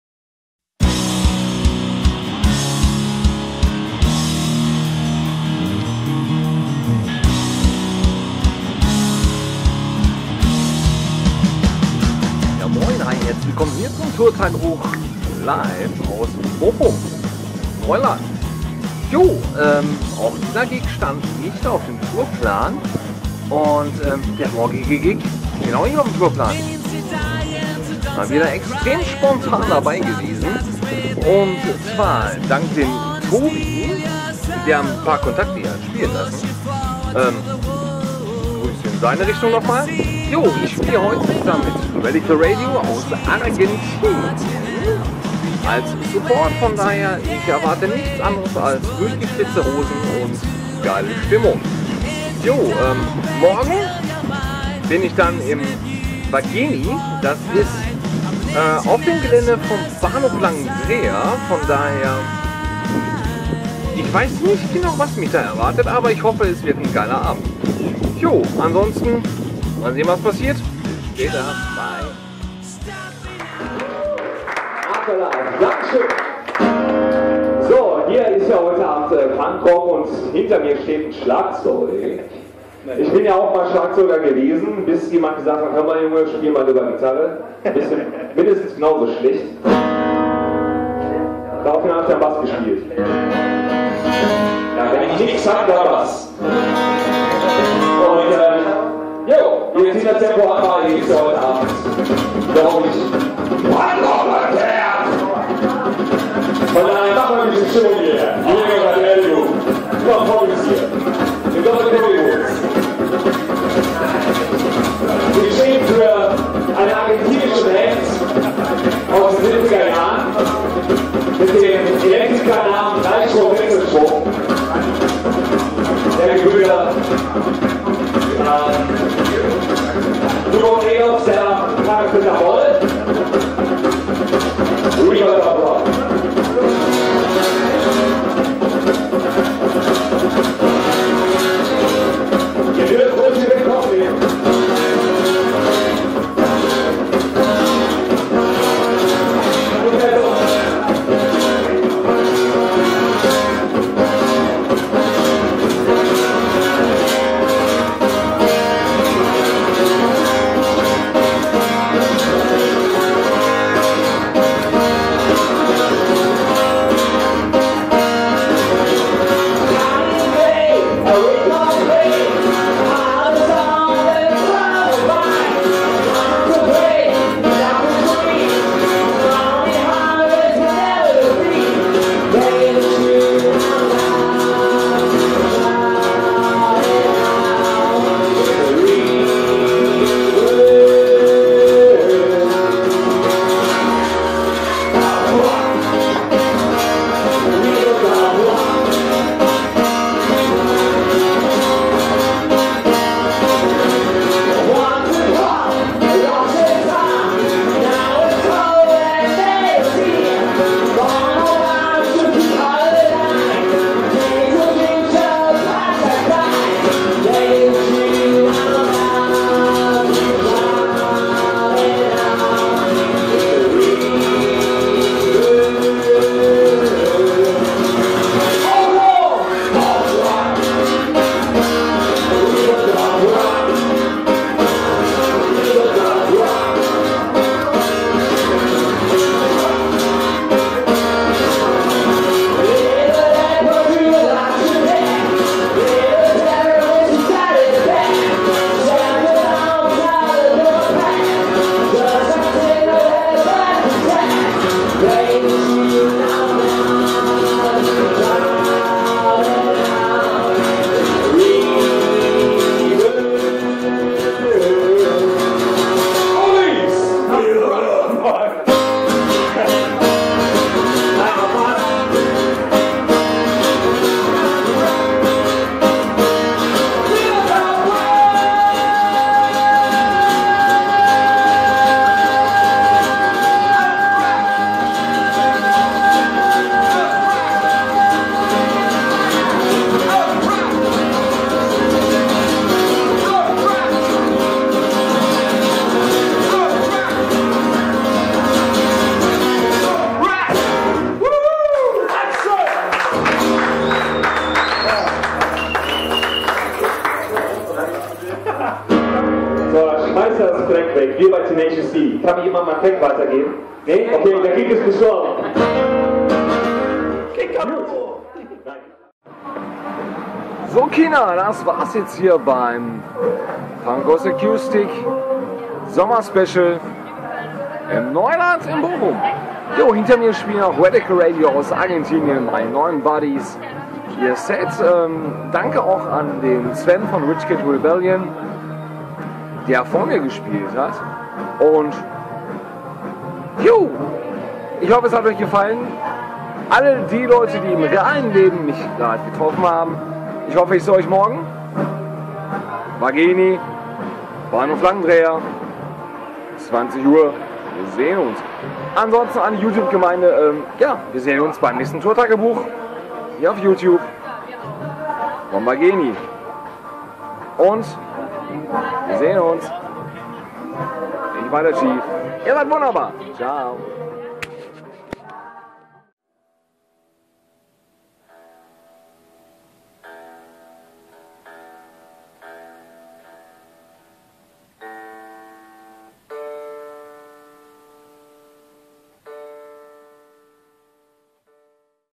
Tourtagebuch Neuland, Bochum 17 5 17
The Chief supported spontan die argentinische Band "Radical Radio" im Neuland, Bochum, im Punk Goes Acoustic - Sommer Special.